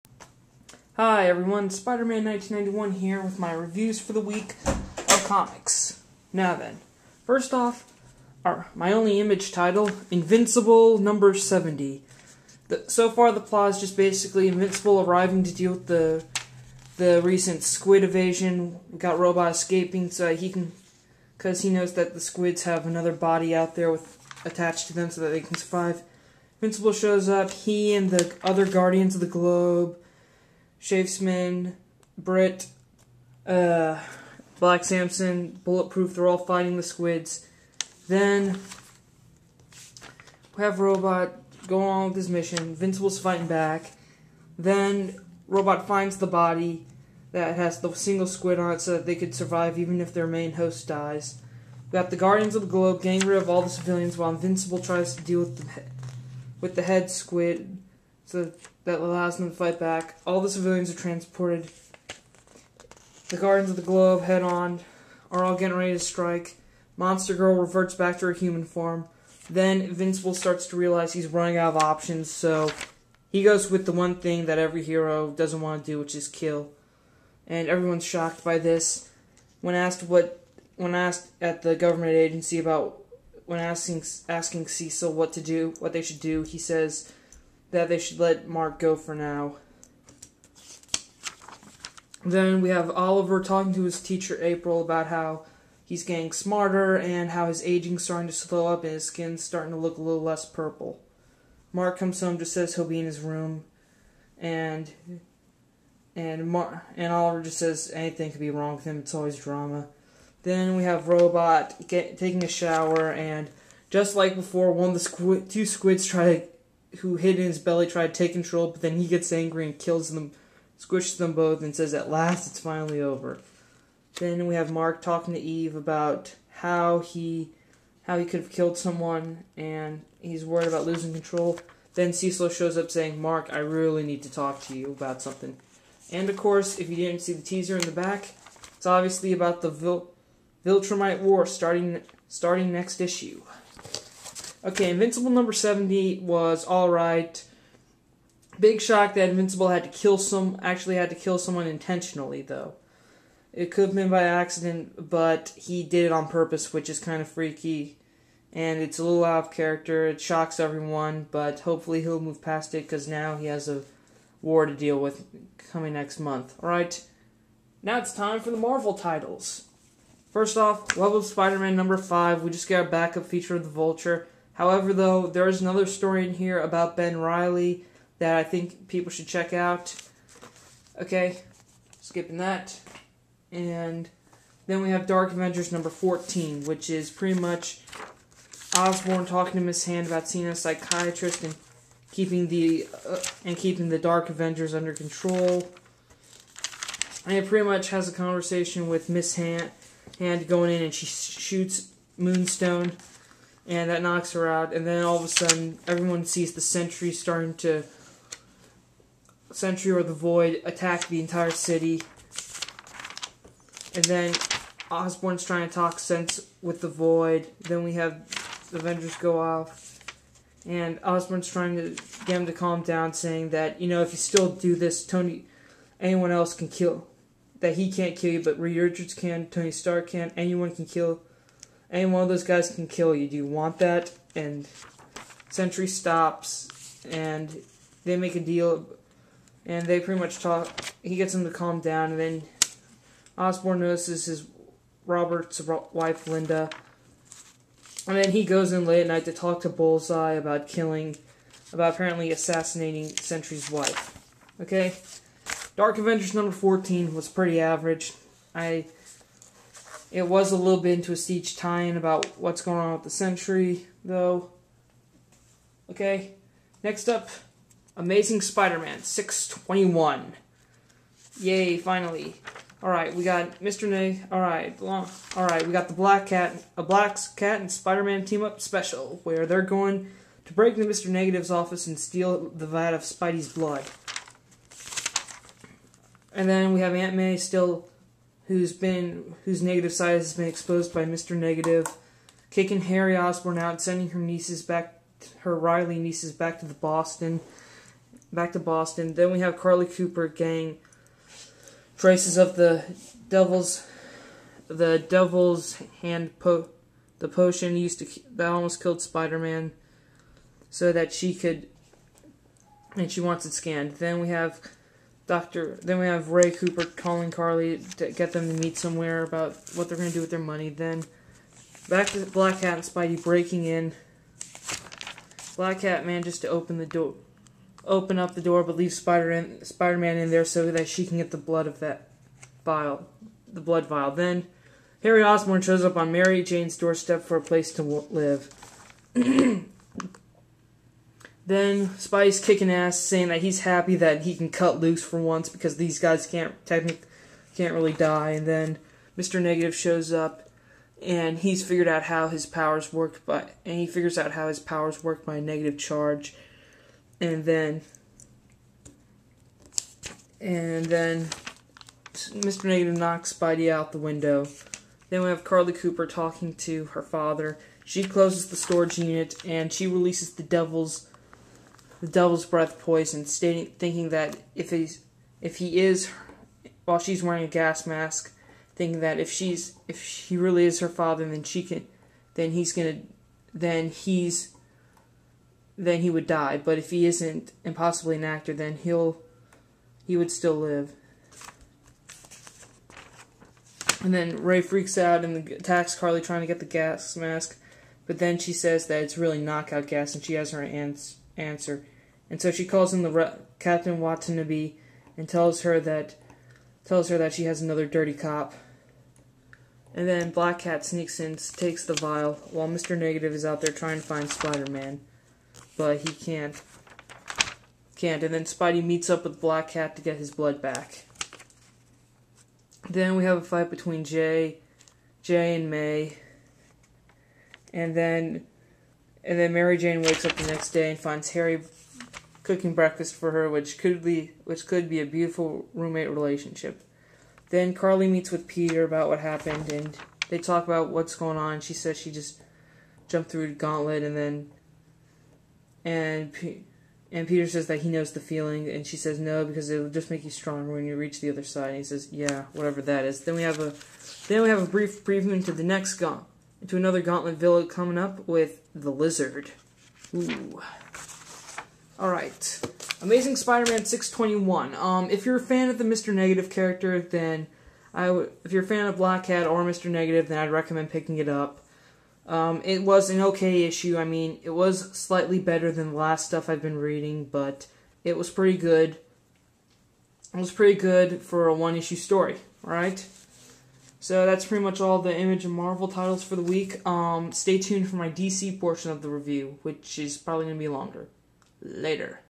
2-17-10 Image and Marvel Comics Reviews
My reviews of Invincible #70, Web of Spider-Man #5, Dark Avengers #14, and Amazing Spider-Man #621. Enjoy!